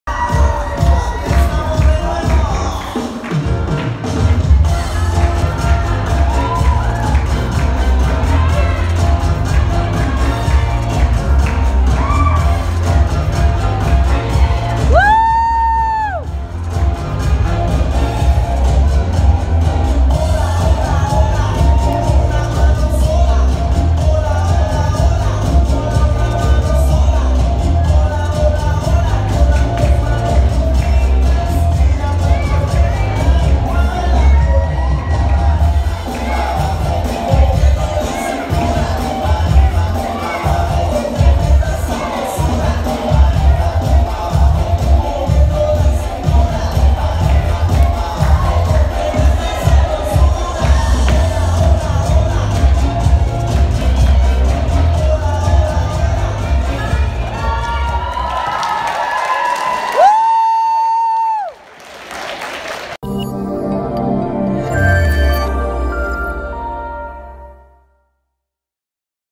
Commonwealth Junior Latin
The Commonwealth Classic 2021
Maeve Corbett
ProAm Junior 1 Gold Cha Cha